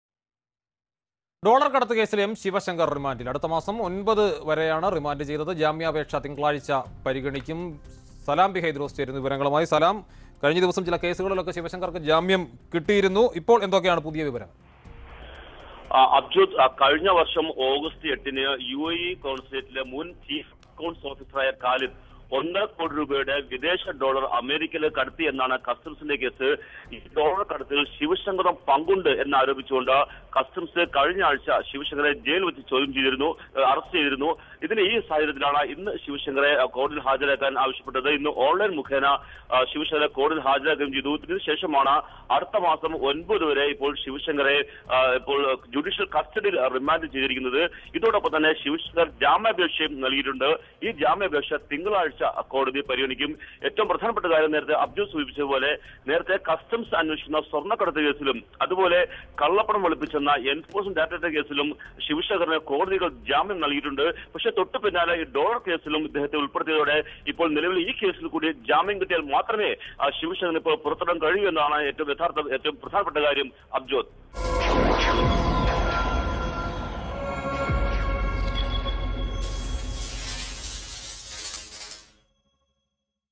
ഡോളര്‍ കടത്ത് കേസില്‍ ശിവശങ്കര്‍ റിമാന്റില്‍;അടുത്ത തിങ്കളാഴ്ച ജാമ്യാപേക്ഷ പരിഗണിക്കും M Sivasankar
ഡോളര്‍ കടത്ത് കേസില്‍ ശിവശങ്കര്‍ അടുത്ത മാസം 9 വരെ റിമാന്റില്‍. അടുത്ത തിങ്കളാഴ്ച ജാമ്യാപേക്ഷ പരിഗണിക്കും